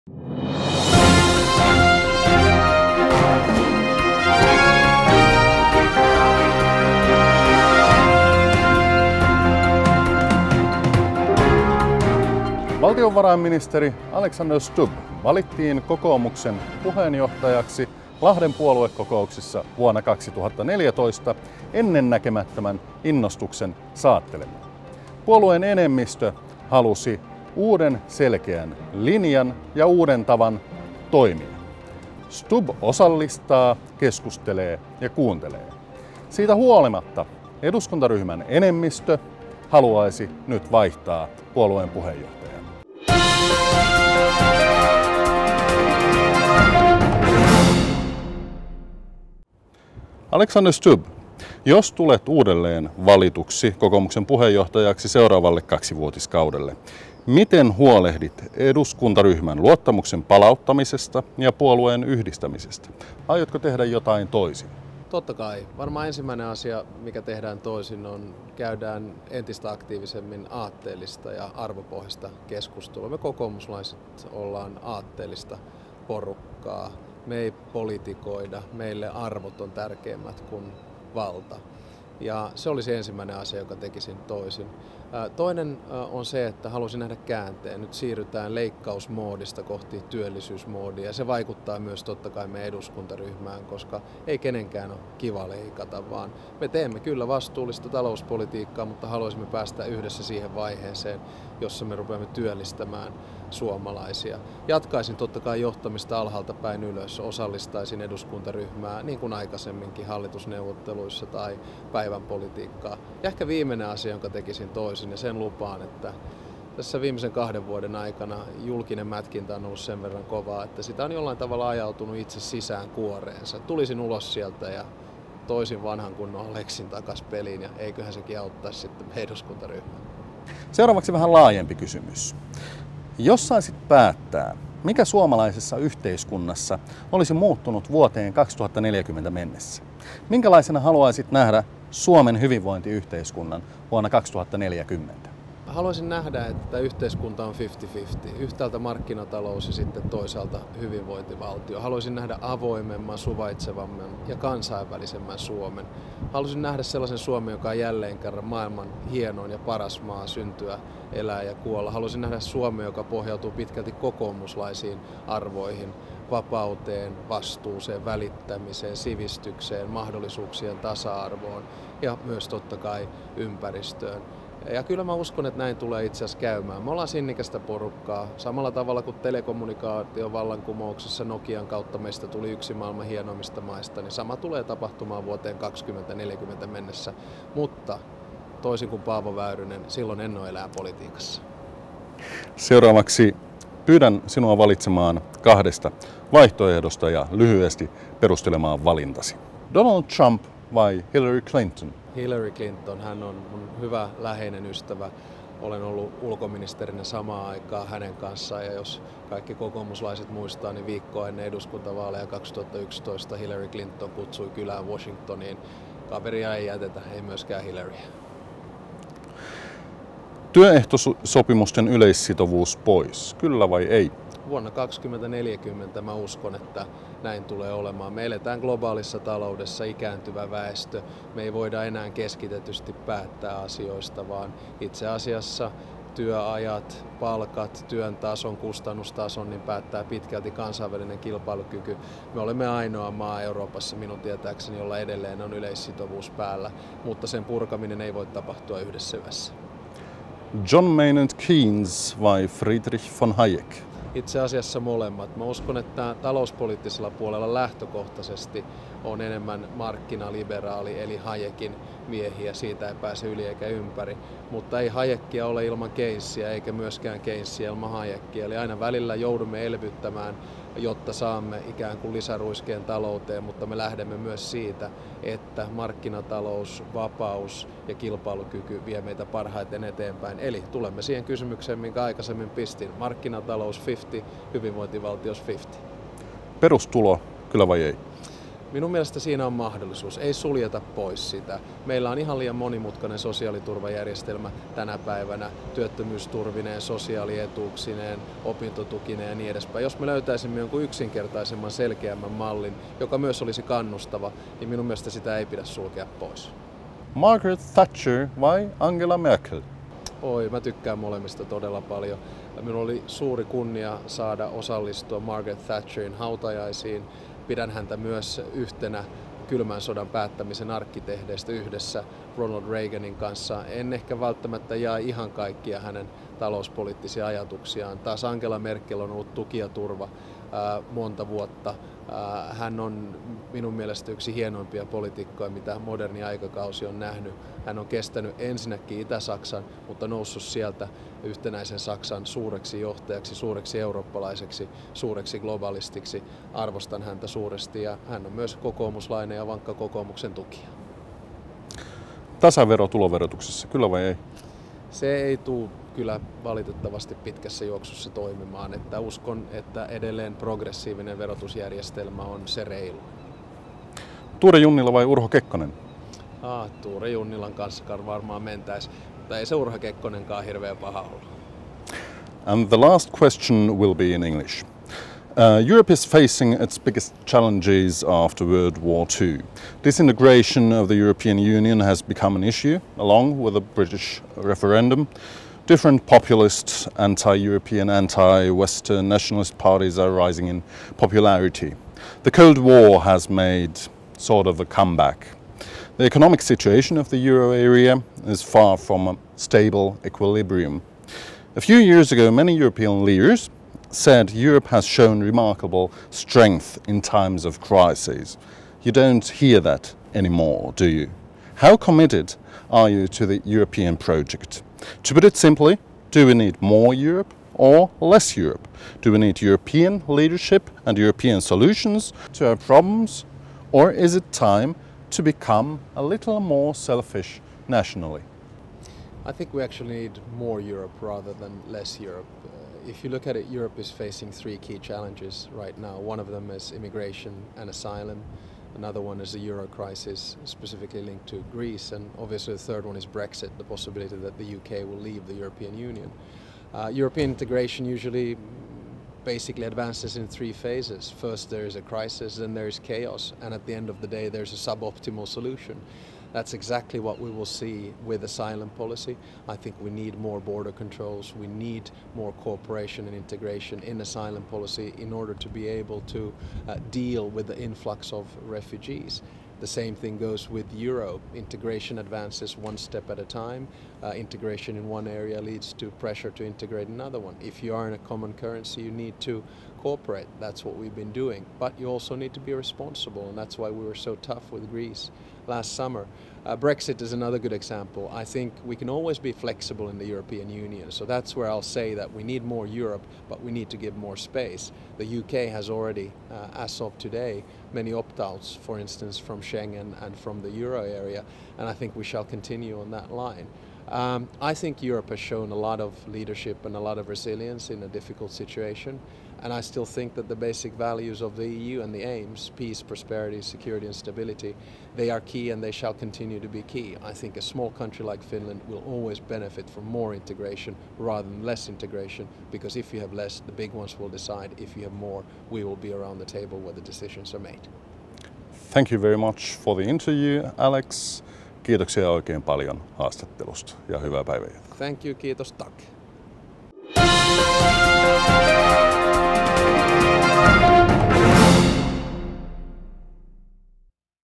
Puheenjohtajaehdokas Alexander Stubb haastattelussa
Verkkouutisten haastattelussa kokoomuksen puheenjohtajaehdokas Alexander Stubb.